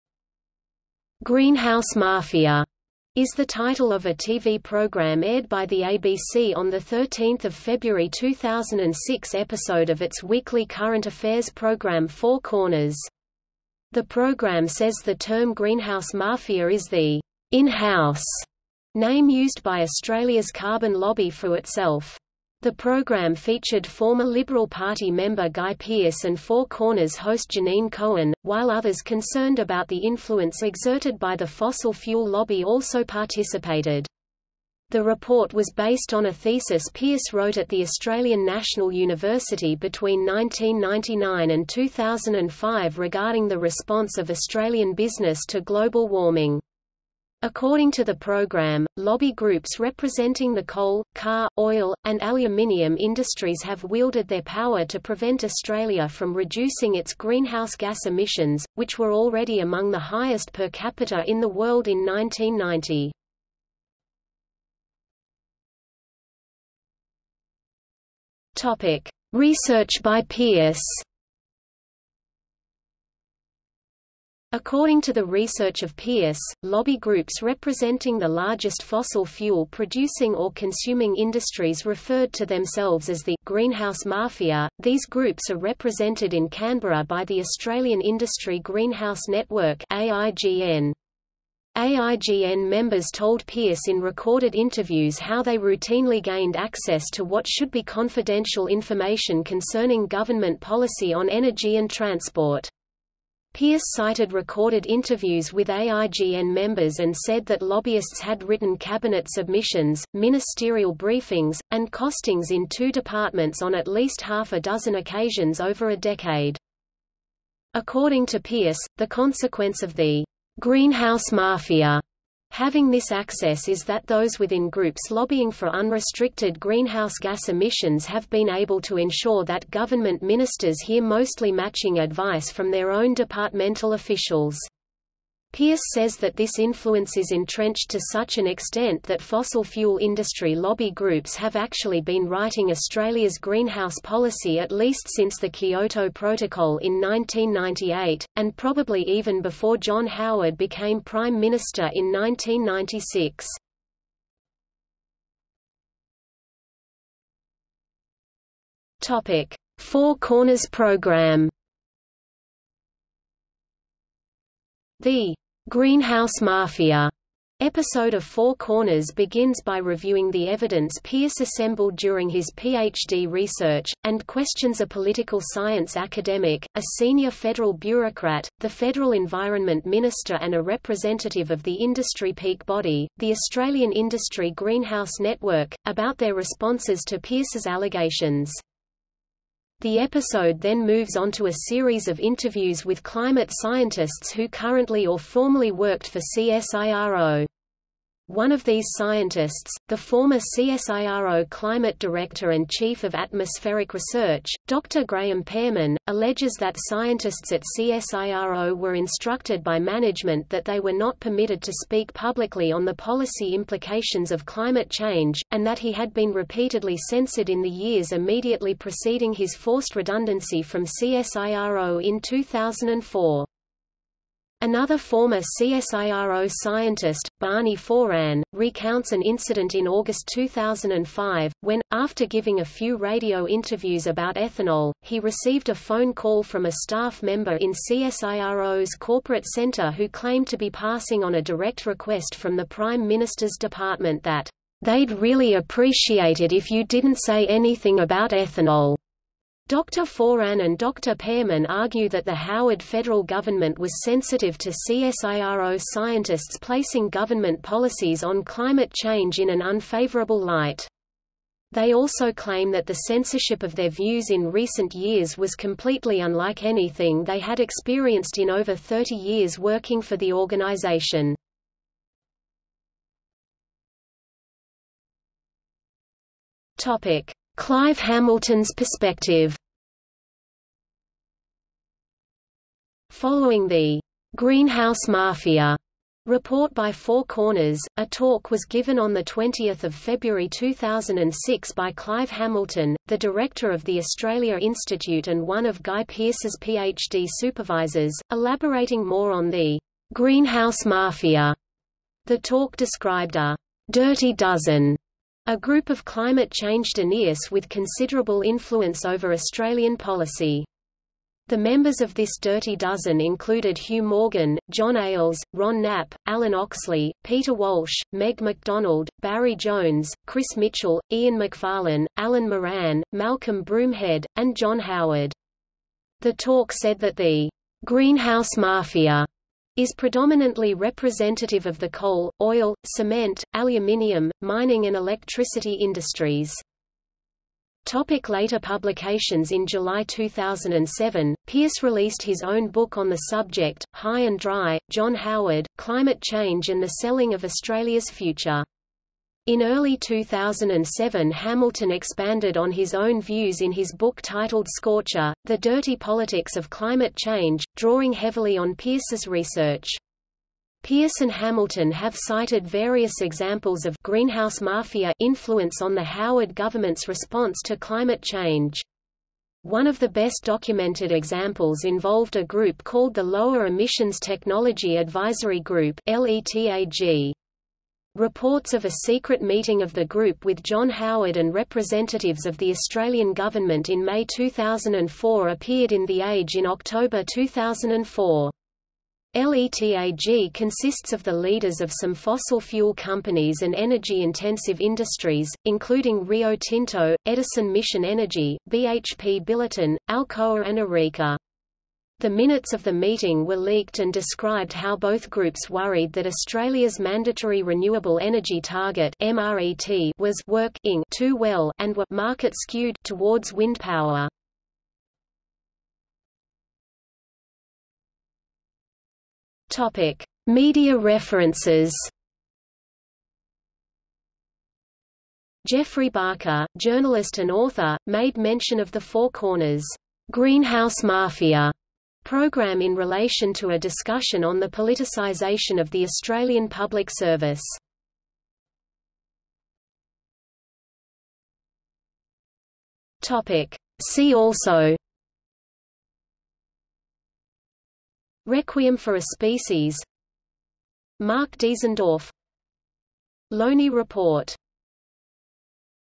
Greenhouse Mafia | Wikipedia audio article
00:01:05 1 Research by Pearse
00:02:35 2 Four Corners program
00:04:33 3 Clive Hamilton's perspective
00:05:37 4 Later publications
00:06:56 5 Media references
00:07:19 6 See also

- Socrates

SUMMARY
"Greenhouse Mafia" is the title of a TV program aired by the ABC on the 13 February 2006 episode of its weekly current affairs program Four Corners. The program says the term greenhouse mafia is the "in house" name used by Australia’s carbon lobby for itself. The program featured former Liberal Party member Guy Pearse and Four Corners host Janine Cohen, while others concerned about the influence exerted by the fossil fuel lobby also participated. The report was based on a thesis Pearse wrote at the Australian National University between 1999 and 2005 regarding the response of Australian business to global warming. According to the program, lobby groups representing the coal, car, oil, and aluminium industries have wielded their power to prevent Australia from reducing its greenhouse gas emissions, which were already among the highest per capita in the world in 1990.